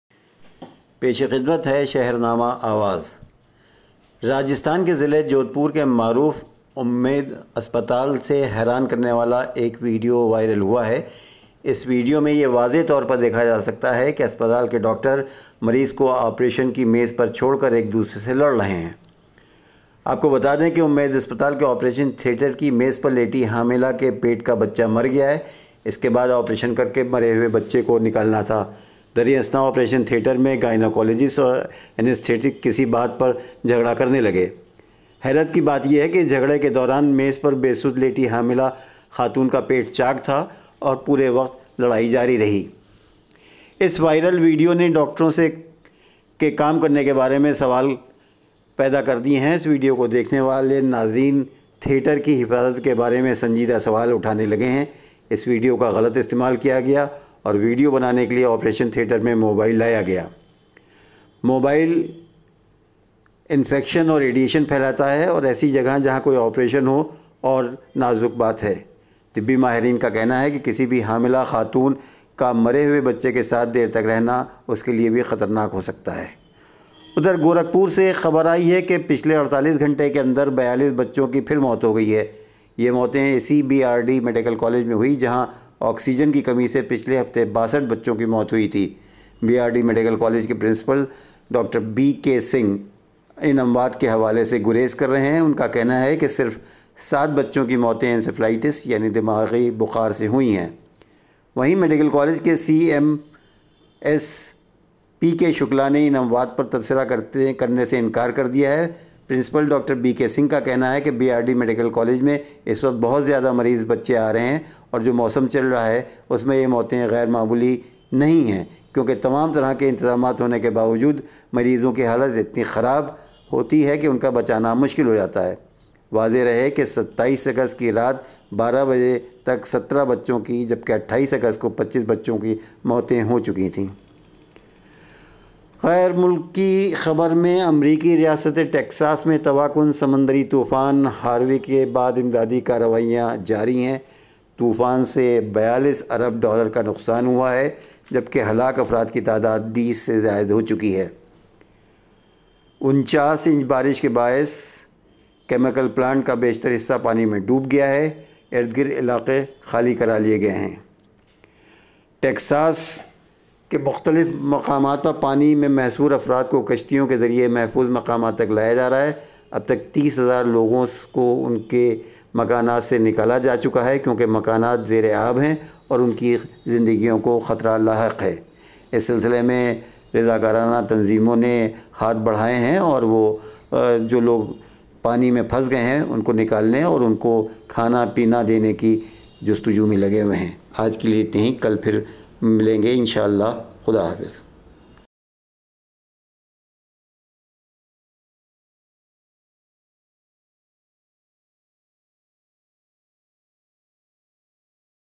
سرجری کے دوران لڑنے لگے ڈاکٹر Shahernama Awaz Urdu News
راجستھان کے ضلع جودھپور کے معروف امید اسپتال سے حیران کرنے والا ایک ویڈیو وائرل ہوا ہے جس میں ویڈیو  یہ واضح طور پر دیکھا جا سکتا ہے کہ اسپتال کے ڈاکٹر مریض کو آپریشن کی میز پر چھوڑ کر ایک دوسرے سے لڑ رہے ہیں.